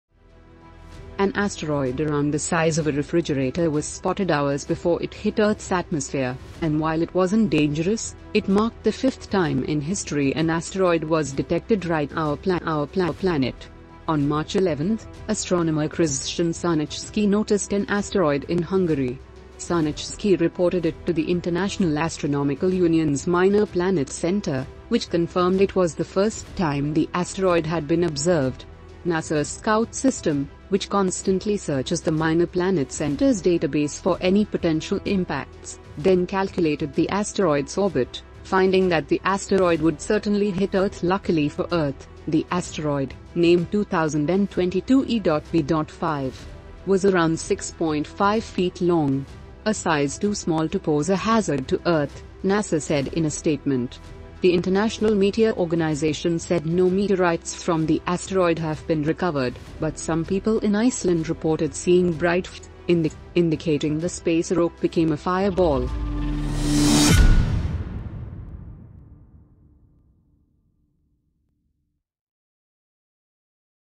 Fridge-sized asteroid hit Earth
Asteroid first spotted two hours before
"Scout" system calculated the asteroid's orbit
NASA confirmed it would certainly hit Earth
Asteroid "too small to pose a hazard to Earth”